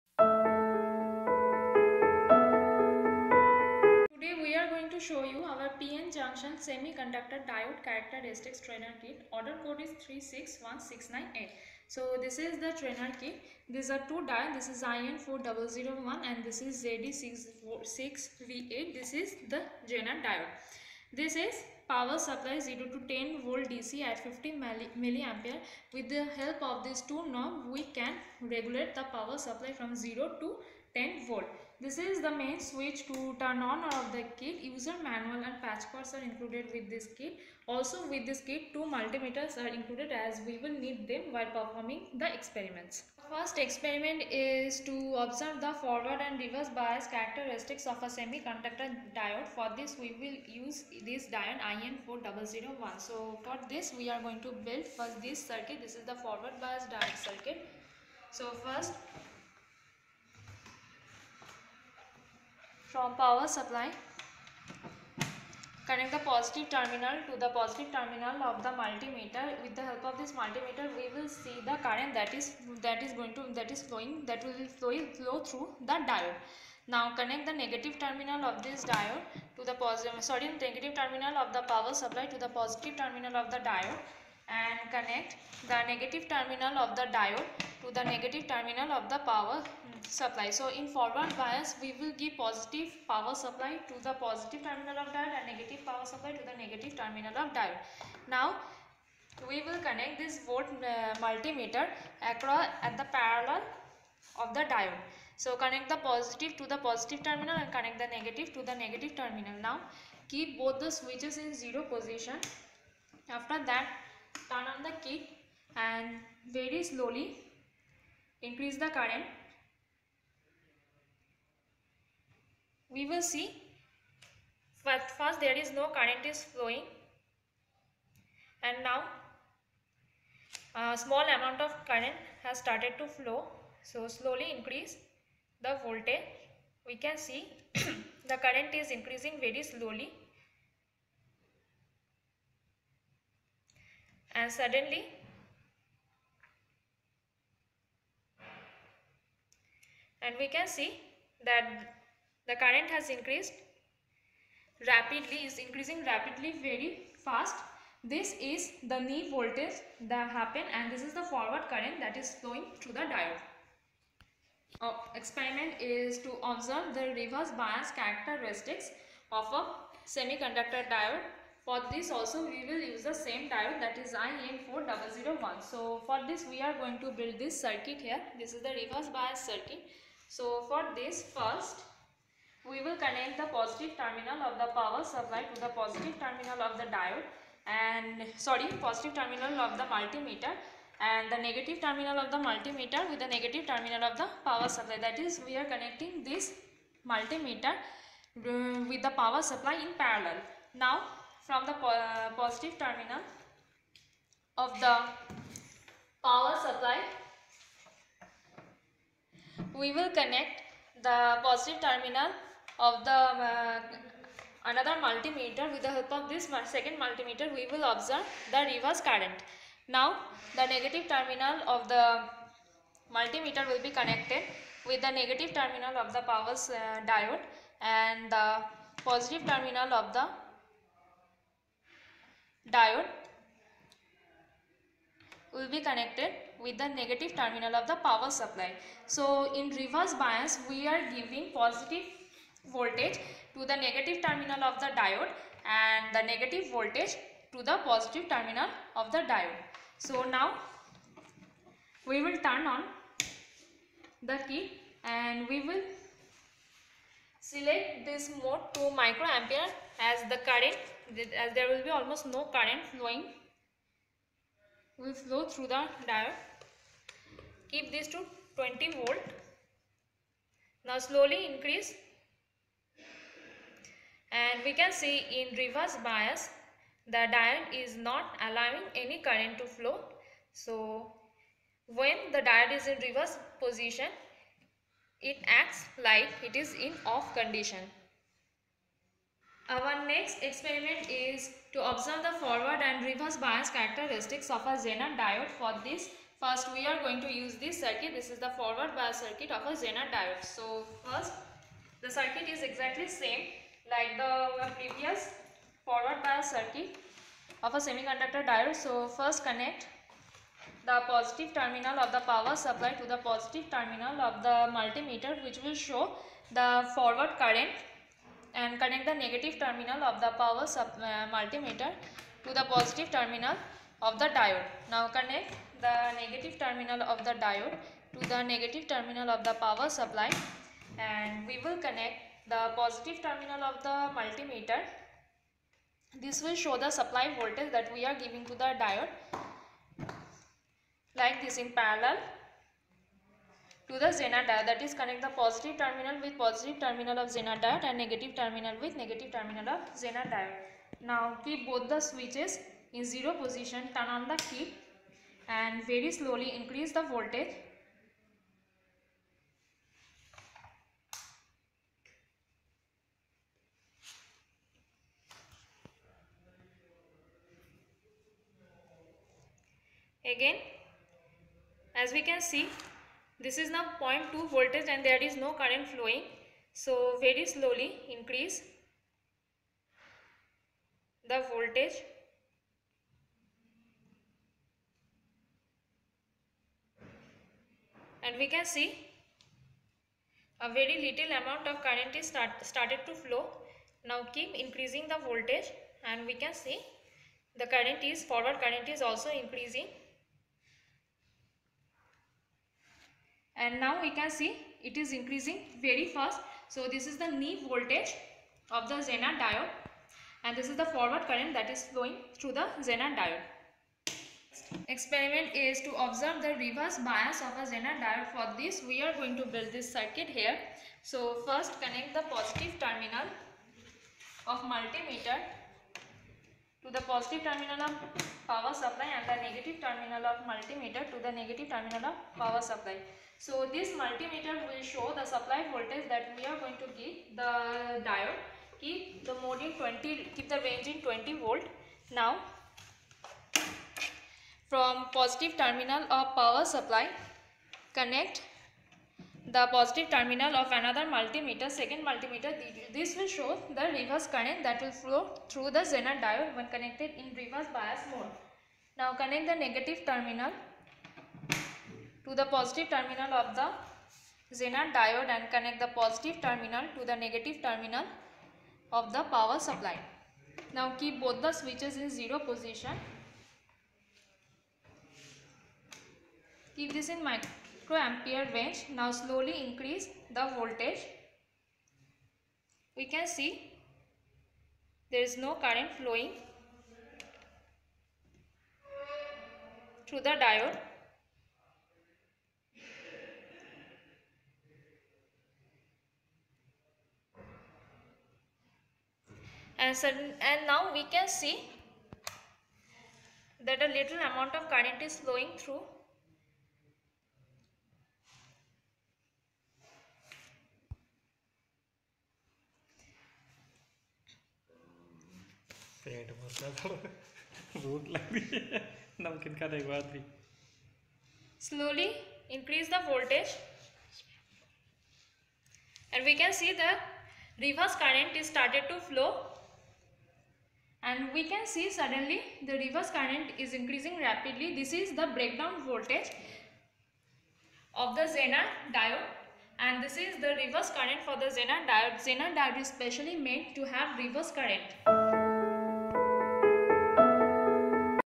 PN Junction / Semiconductor Diode Characteristic Apparatus || Tesca 36169A
PN_Junction / Semiconductor Diode Characteristic Apparatus

In this video we described about PN Junction / Semiconductor Diode Characteristic Apparatus.